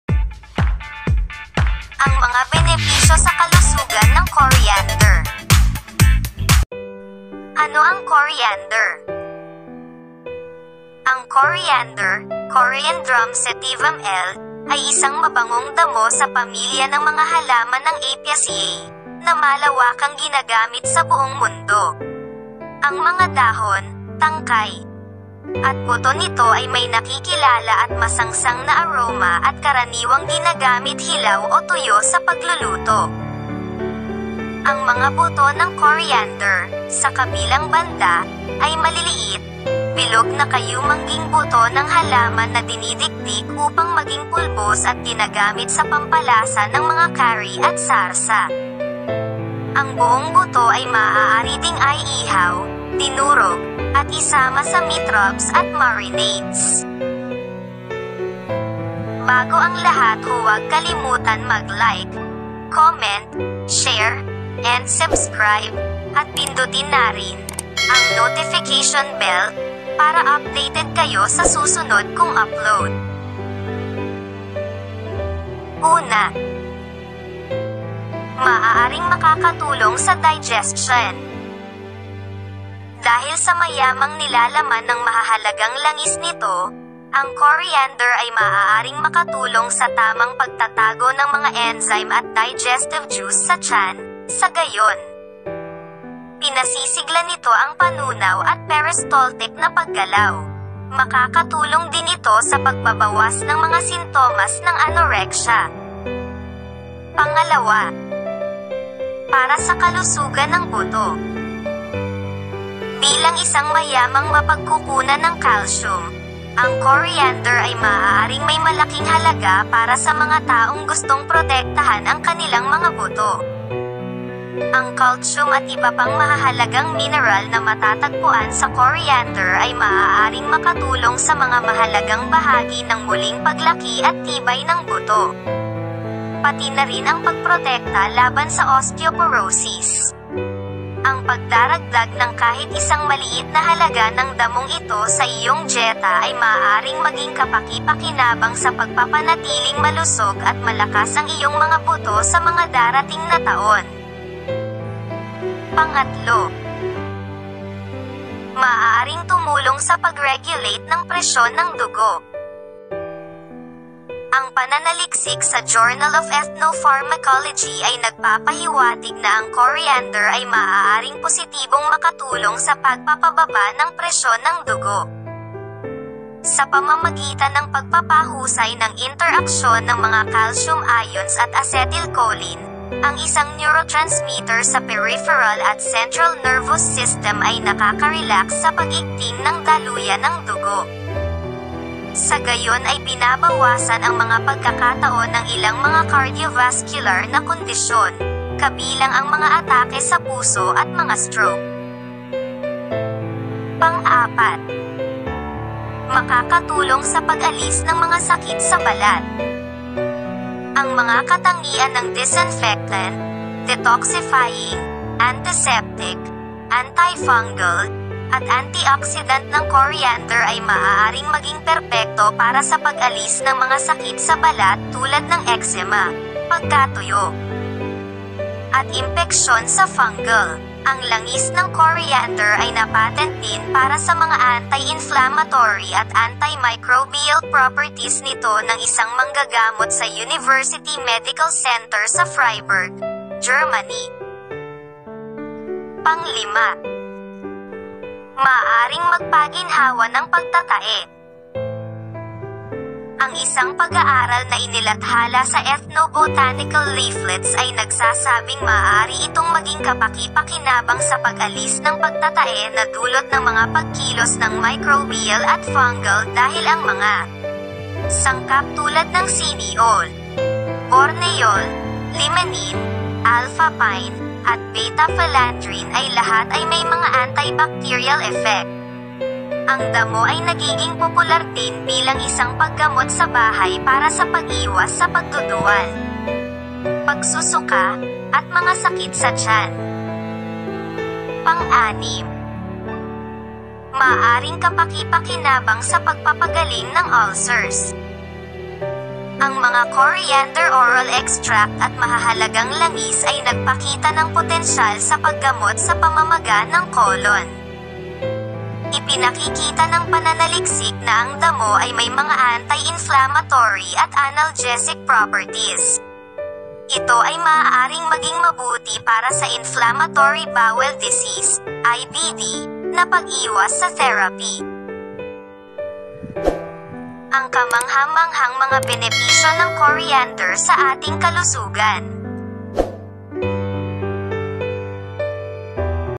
Amazing Benefits of Coriander Seeds | Health Tips | Promote bone health | Natural Health Benefits
Coriander is known worldwide for its medicinal properties. It is a great source of potassium, iron, vitamins A, K, and C, folic acid, magnesium, and calcium can cure many health issues.

 Is a spice and medicinal plant in the Umbrella family. This family includes: parsley, dill, fennel, carrots, celery, parsnip, etc. Coriander has a second name - cilantro, called coriander vegetables. The word “cilantro” is more commonly used in colloquial speech. It is both spice and herb at the same time..